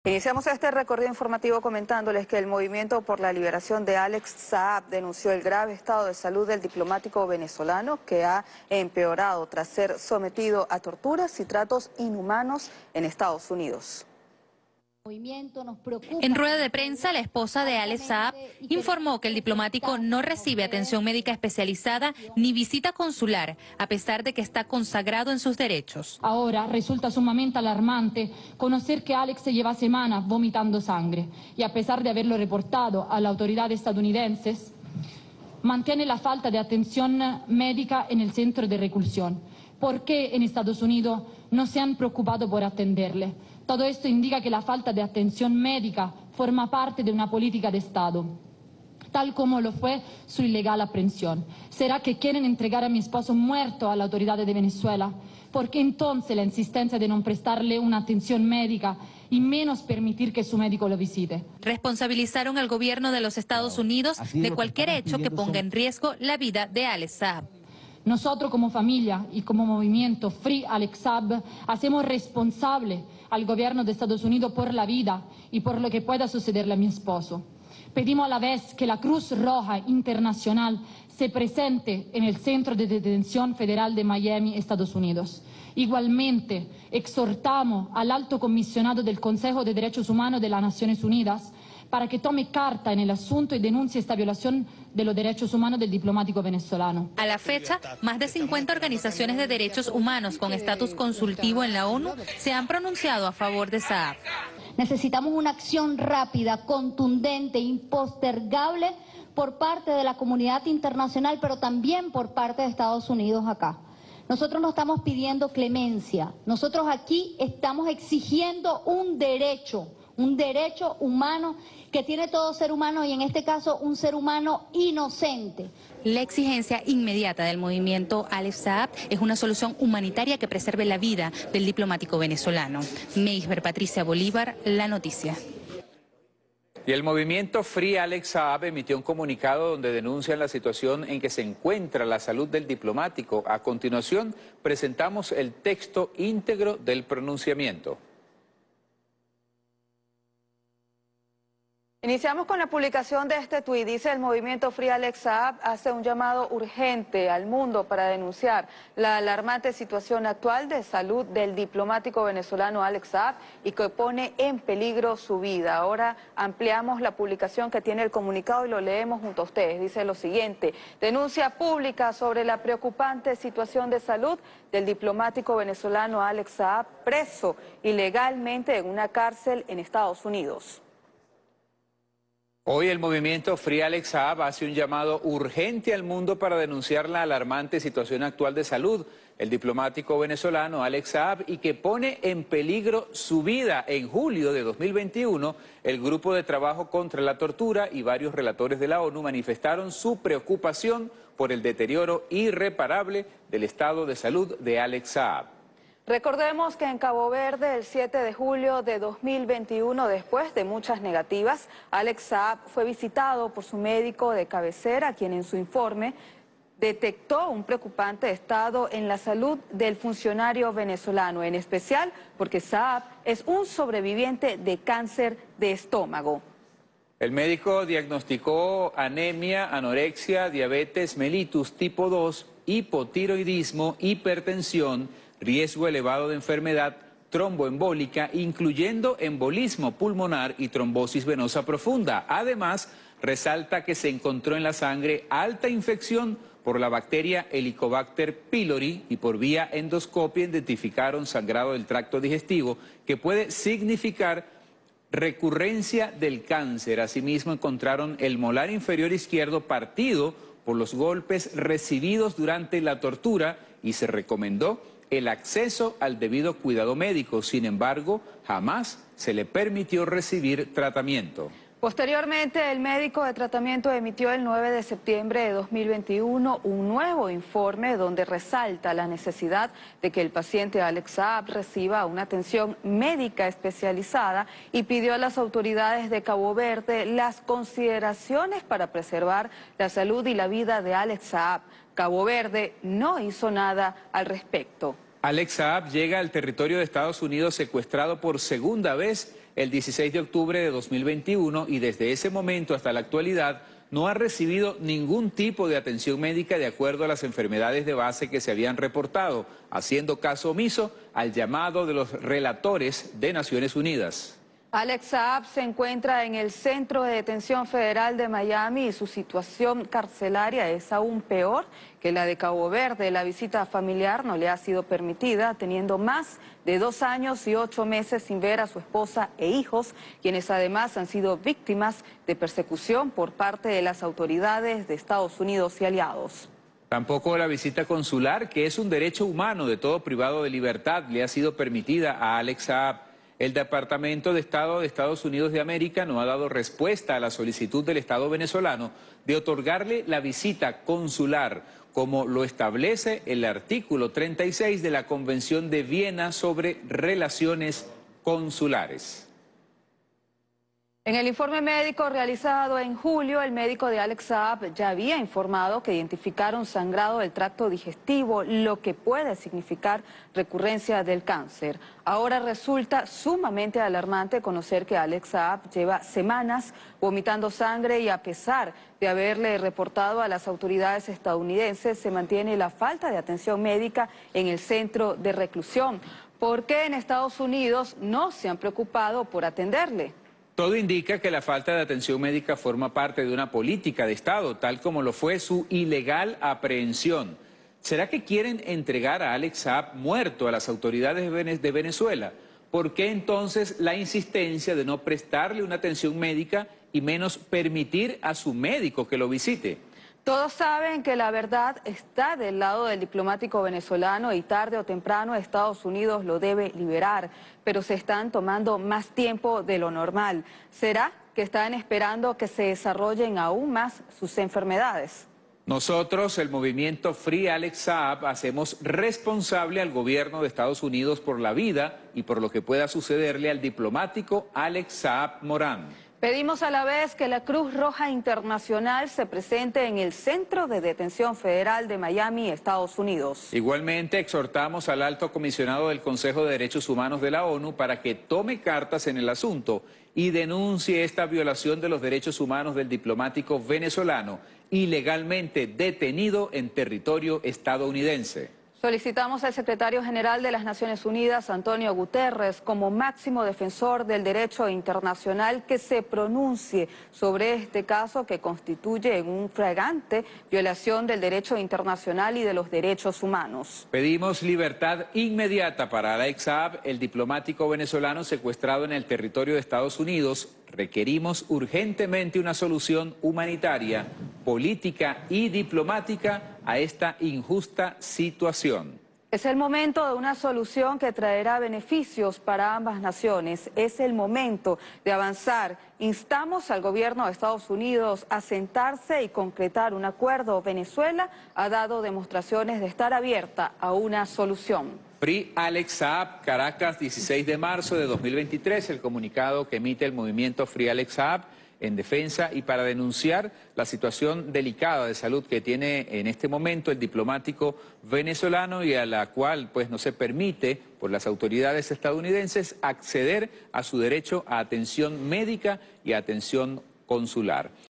Alex Saab está vomitando sangre (es sobreviviente de cáncer de estómago) y le niegan atención médica
La esposa de Alex Saab, Camilla Fabri de Saab, y el Moviminto Free Alex Saab, denuncian este 16 de marzo de 2023 que se le ha negado atención médica especializada en su prisión en Estados Unidos, dado que él es sobreviviente de cáncer de estómago y el mismo, al parecer, podría estar recurriendo, pues desde hace semanas ha estado vomitando sangre. Emiten un comunicado exigiendo darle la atención adecuada, así como ser visto por la Cruz Roja y el Alto Comisionado de Derechos Humanos. Recuerdan que él es un funcionario diplomático venezolano y su prisión es ilegal.

USDT o Bitcoin (BEP20): 0xef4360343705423a24119205eeb1de2a10fd23f4

USDT (TRC20): TQJB86JZjF5Kt1C7f322aax4iyD6x65UcV

Petros: PFfDUW3cj1WD7cUPYg9MK2EtgG5nUgVzuc